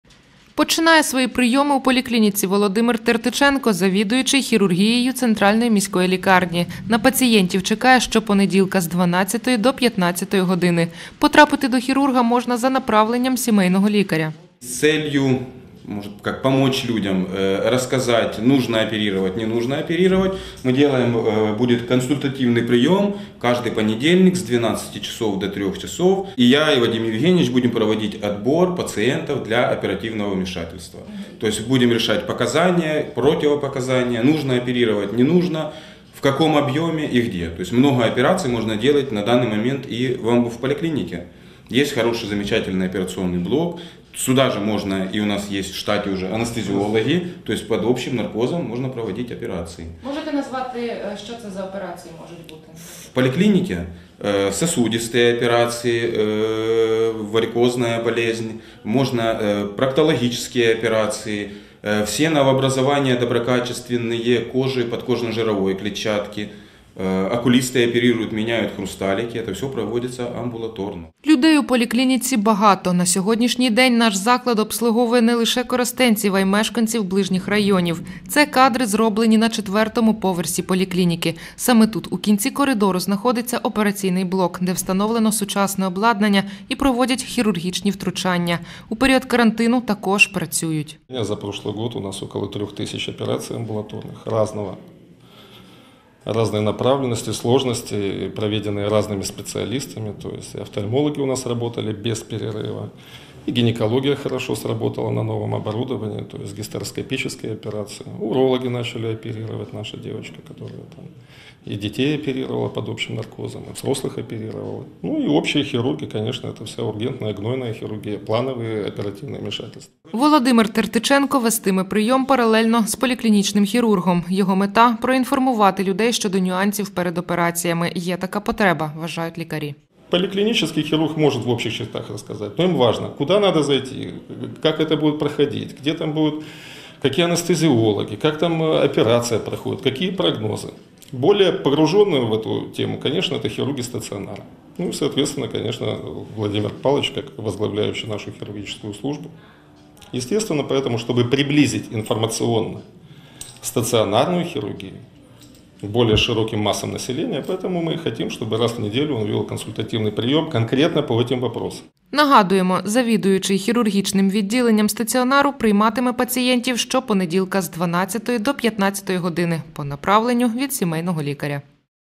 KorostenTV_31-08-21_Починає прийоми у поліклініці Володимир Тертиченко.
Починає свої прийоми у поліклініці Володимир Тертиченко, завідуючий хірургією центральної міської лікарні. На пацієнтів чекає щопонеділка з 12:00 до 15:00. Потрапити до хірурга можна за направленням сімейного лікаря.